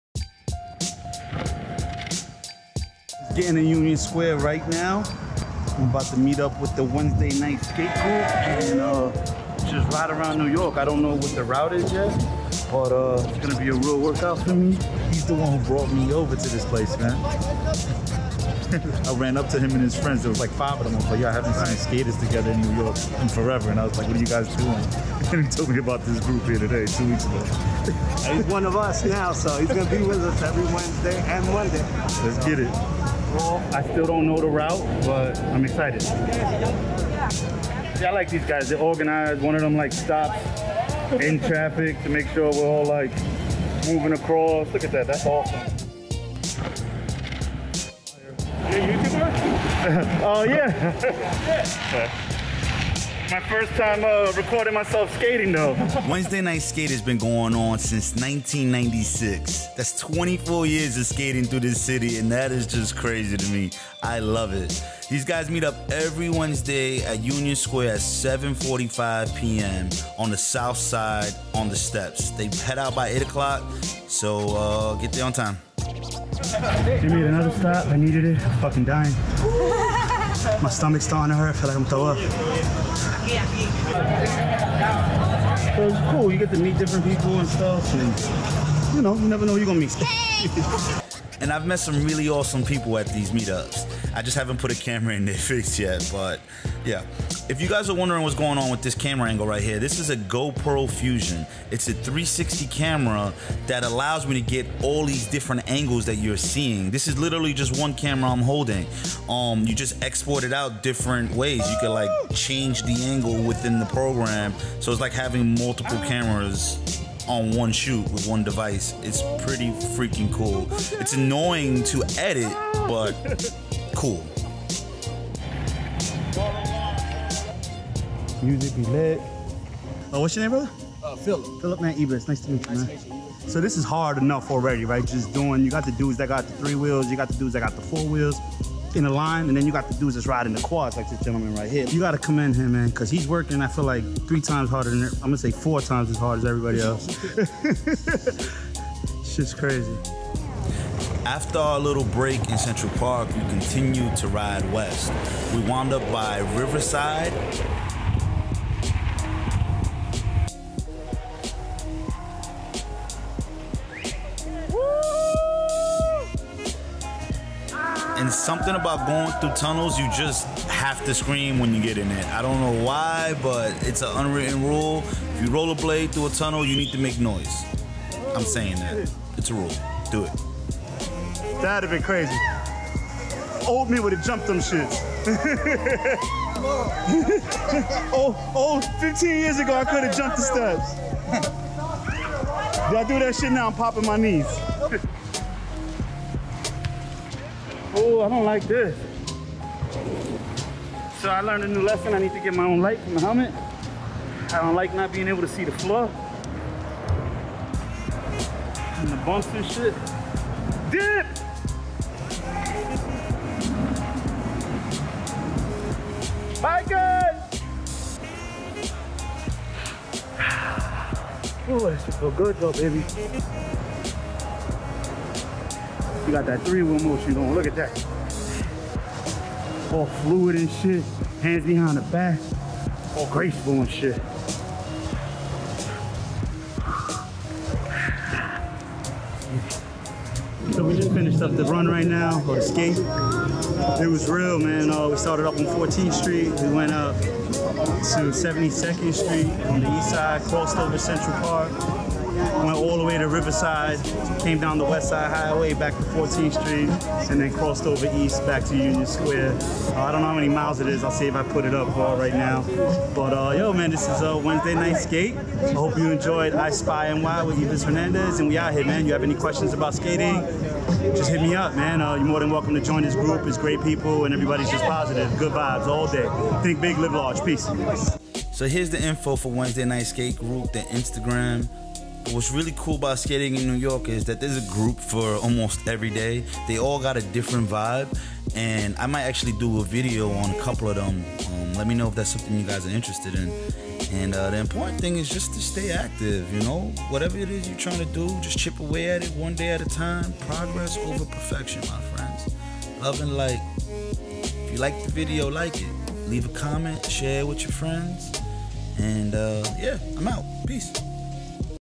Skating in New York (Let Me Put You On Ep 2)
In this vlog we go skating thru the streets of New York City with the Wednesday Night Skate crew. They have been organizing this weekly skating  meet up in New York City since 1996. Check them out on instagram at @wednesdaynightskate. The meet ups are at 14 St. Union Sq. (at the steps)  every Wednesday at 7:45 PM and they head out by 8PM.

Monday Night Skates

Empire Skate Group

CIB NYC

Thanks for the Love.Think Big. Live Large.